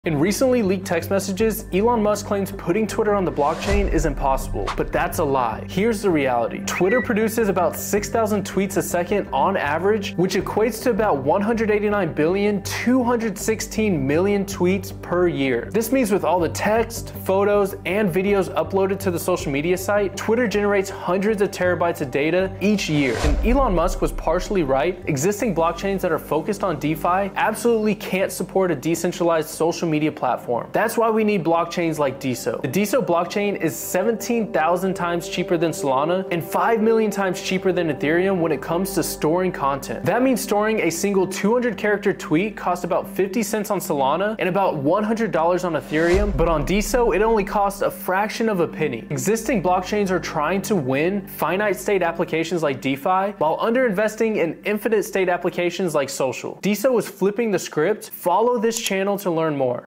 Elon Musk is Wrong About Twitter
In recently leaked text messages Elon Musk claims putting Twitter on the blockchain is impossible, but he is wrong. Here’s the truth:

Twitter gets about **6,000 Tweets a second** on average, give or take which equates to about **189,216,000,000 Tweets per year. This means with all of the text, photos, and videos uploaded to the social media site, Twitter generates hundreds of terabytes of data each year.**

And Elon was partially right, e**xisting blockchains that are focused on DeFi absolutely can’t support a decentralized social media platform. That’s why we need blockchains like DeSo.**

The DeSo blockchain is **~17,000 times cheaper than Solana**, and **~5 million times cheaper than Ethereum** when it comes to **storing content**. To put that in terms that are easier to understand, **storing a single 200-character "Tweet"** costs about **$0.50 on Solana**, about **$100 on Ethereum**, but only **$0.00017 on DeSo**

**Existing blockchains are trying to "win" finite-state applications like DeFi, while under-investing in "infinite-state" applications like social.**

DeSo is flipping the script, follow this channel to learn more.